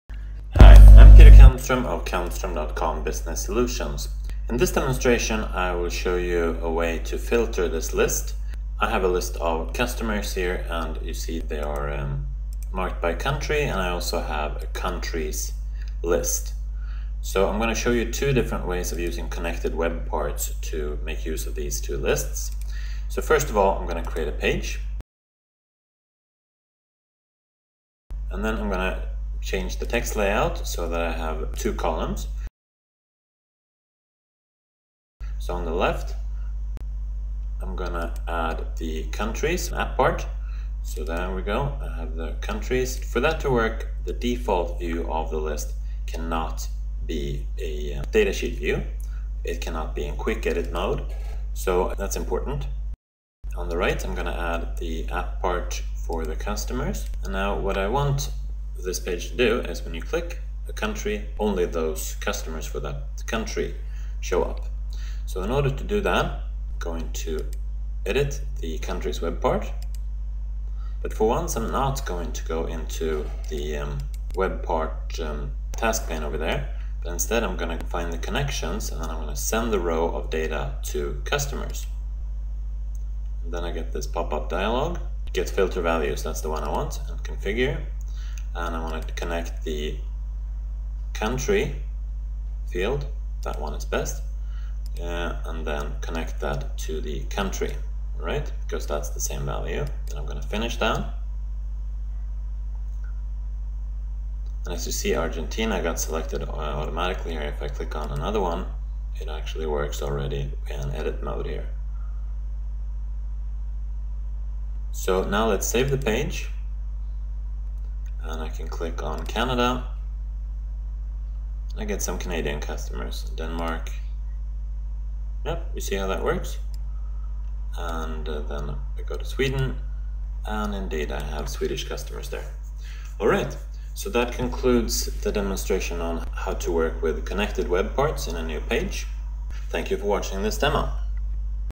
Connect SharePoint App Parts and Click to Filter
Use SharePoint app parts to connect a Countries list to a Customers list and click to filter